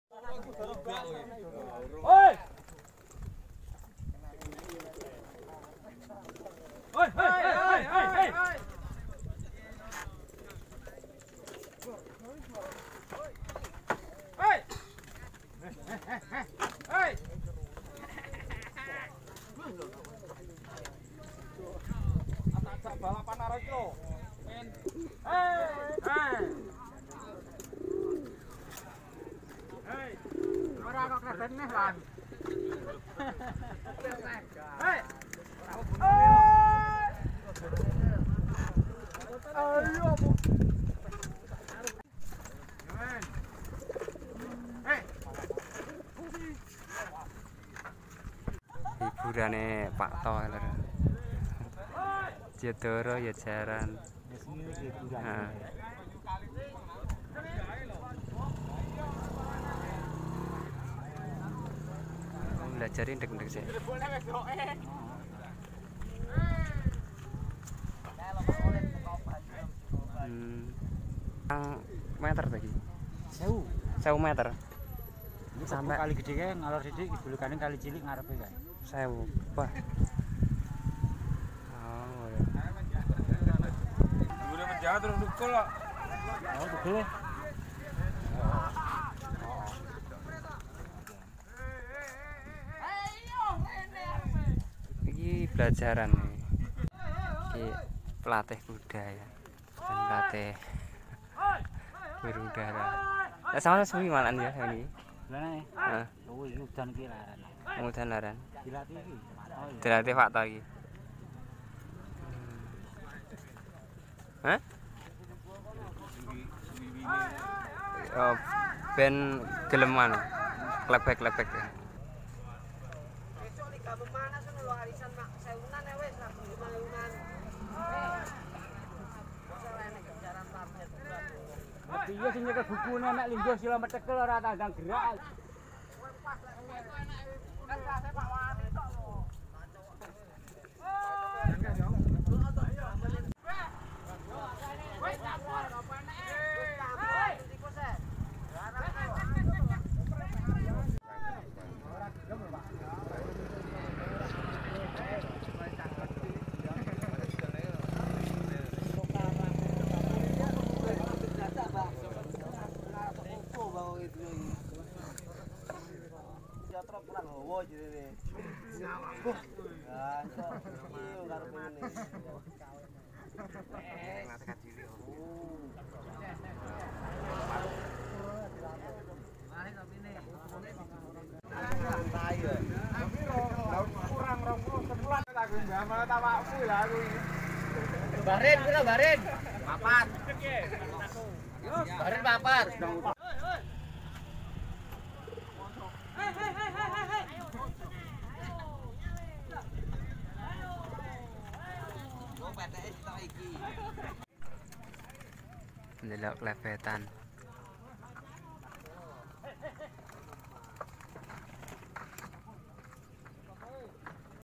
Serunya Suasana Balap Merpati || Latber Di Pikatan
Melihat Balap Merpati || Bersama Pak To Kru Nona Cindy Pikatan.